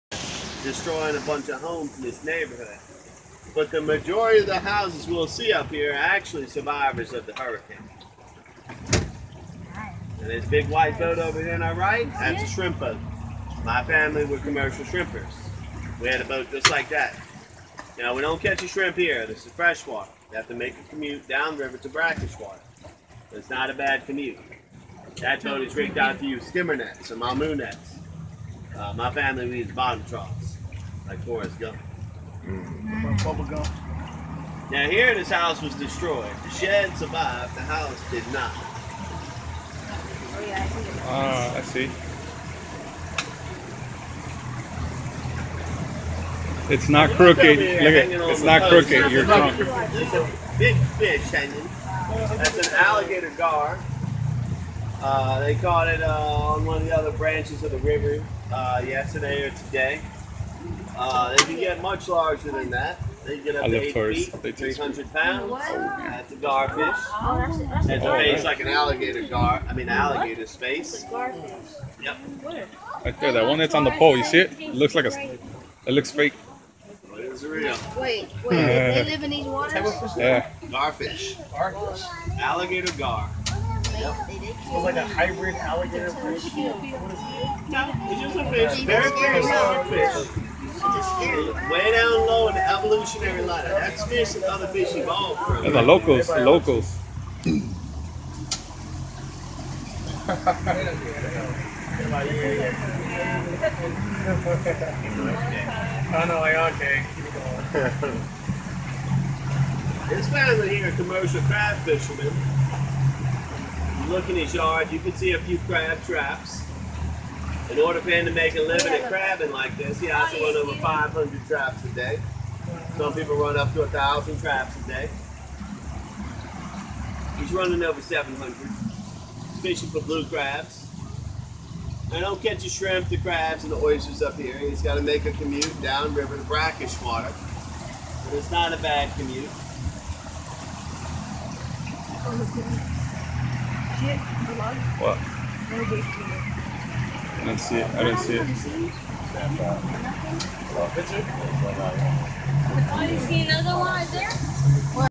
Gator tour in New Orleans Swamp Tour boat 2015.
Bubba Gump Shrimping boat is discussed by the tour guide. fun trip. take a boat ride with me.